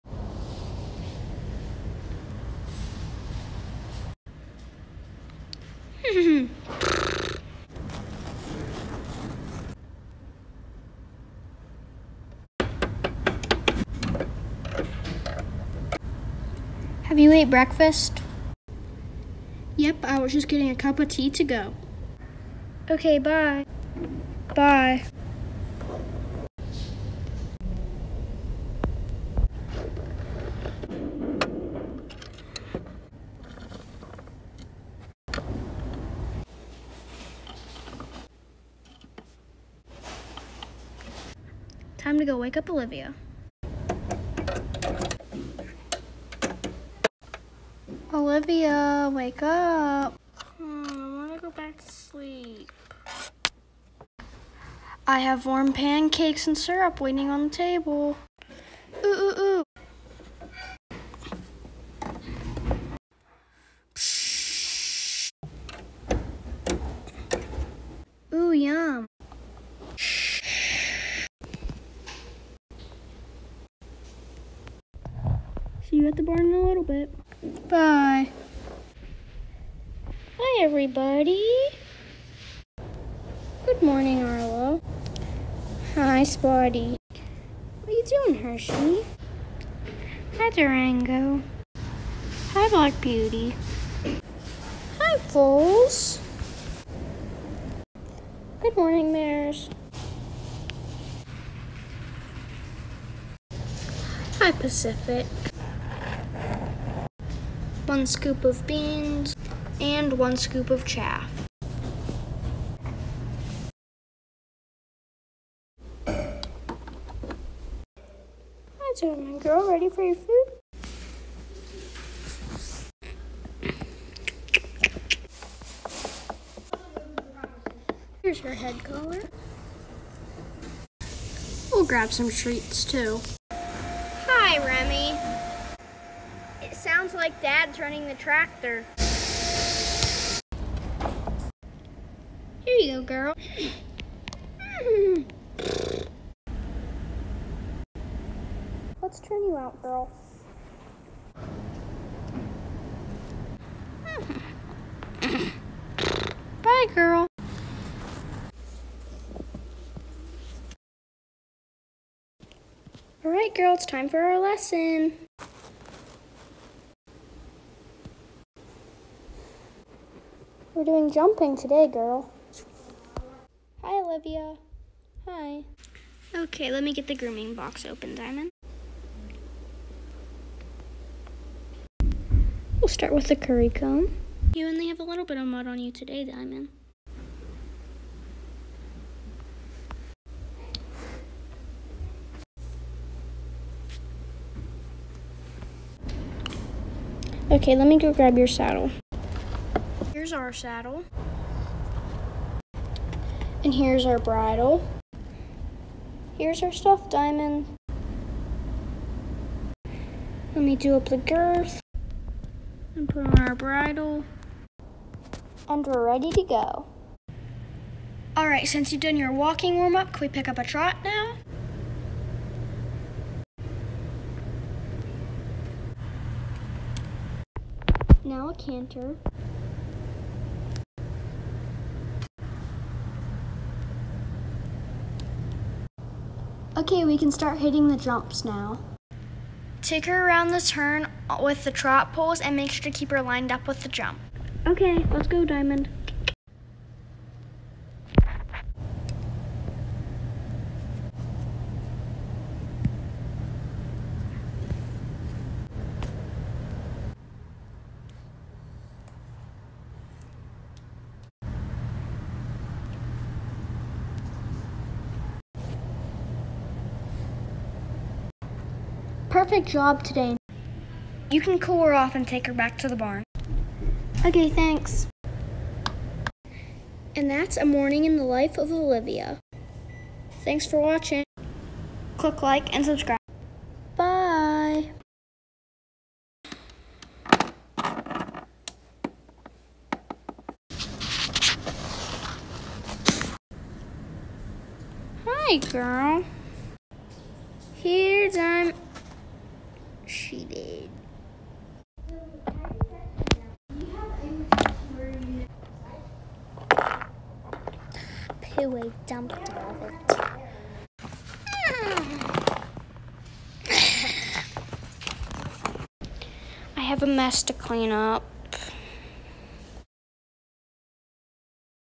A Morning In The Life Of Olivia | A Schliech Episode | Brenna’s Adventures
Olivia is Abby and John’s daughter and she is taking care of her horse on their family’s horse stable Champion Rider Stables.